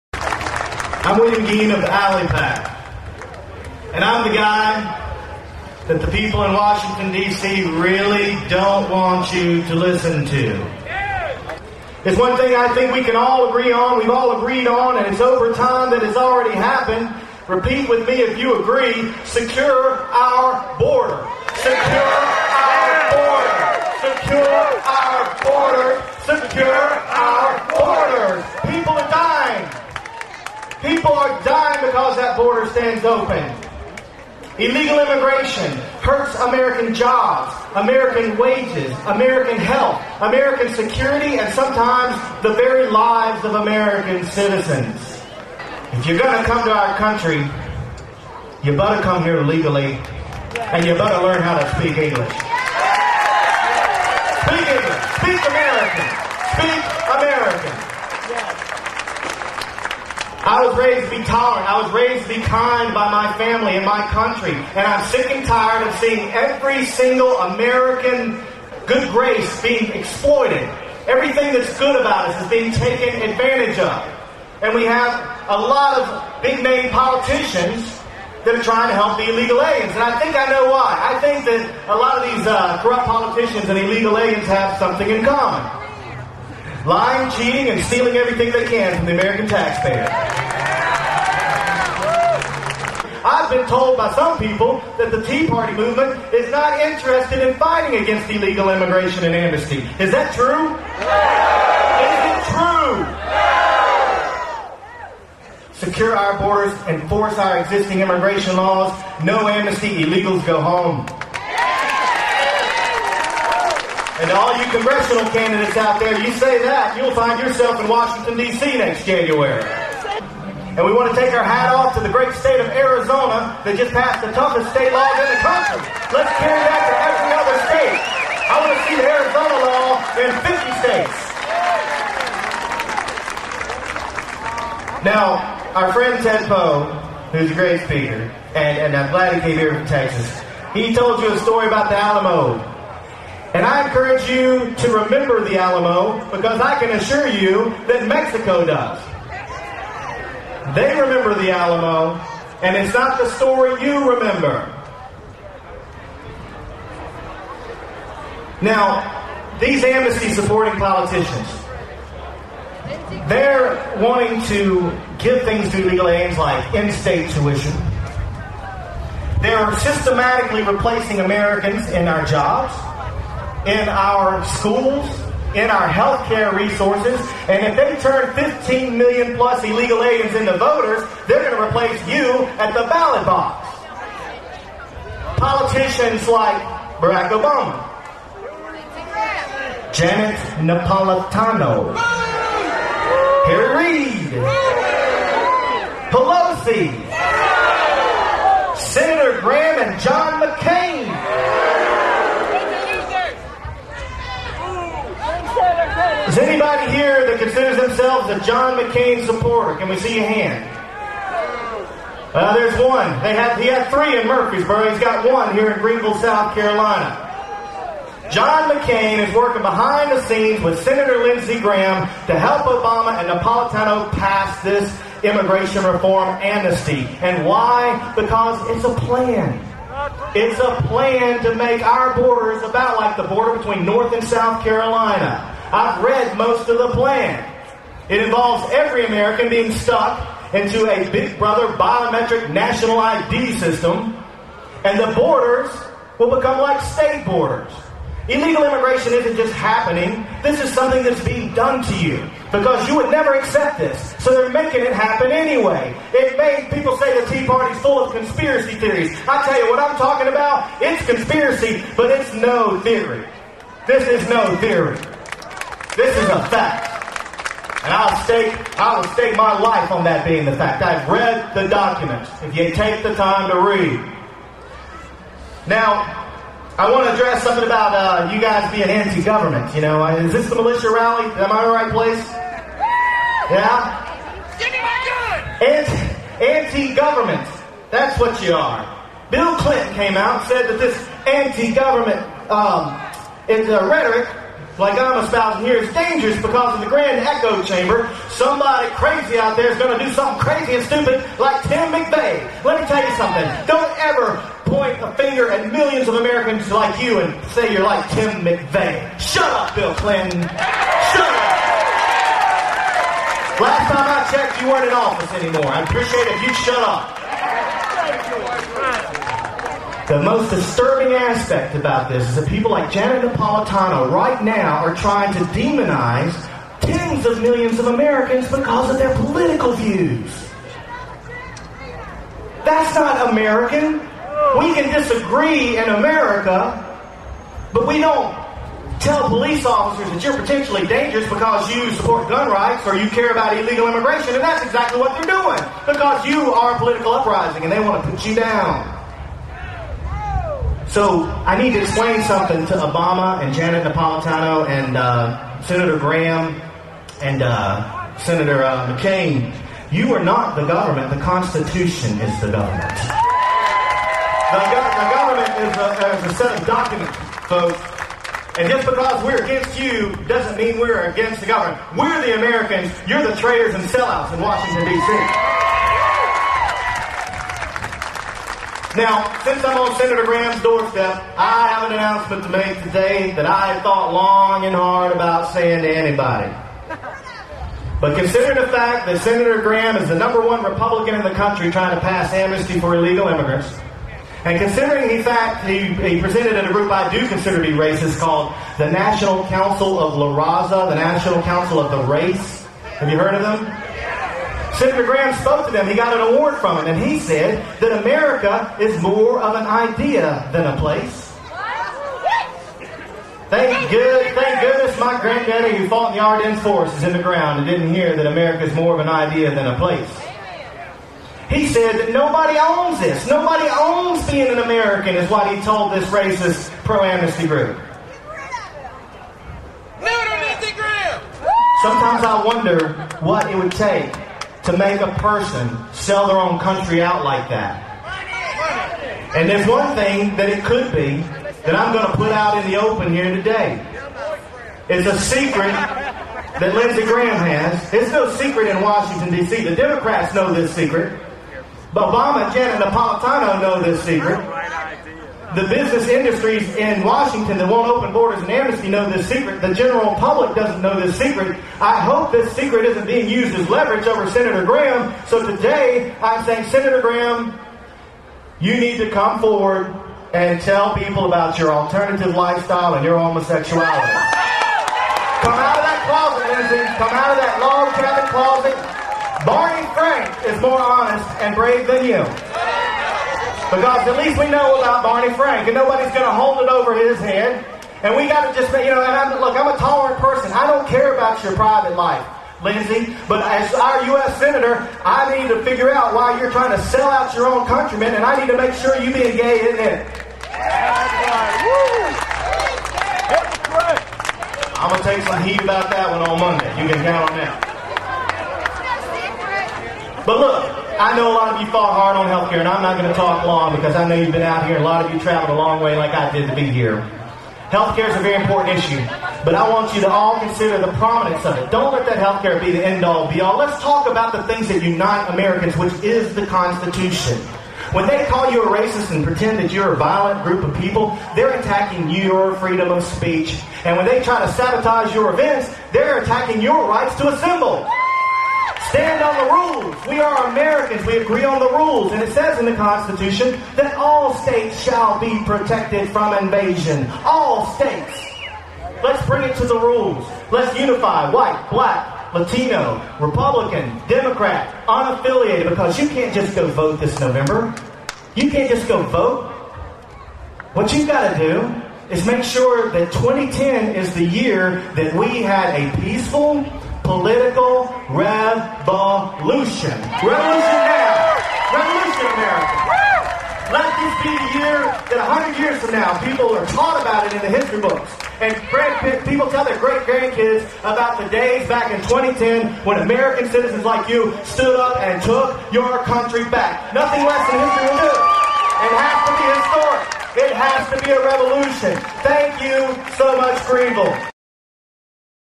US Senator Graham Tell About Homosexuality
At Tea Party in Greenville SC, William Gheen, President of ALIPAC, urges Senator Lindsey Graham to tell South Carolinians about his homosexuality.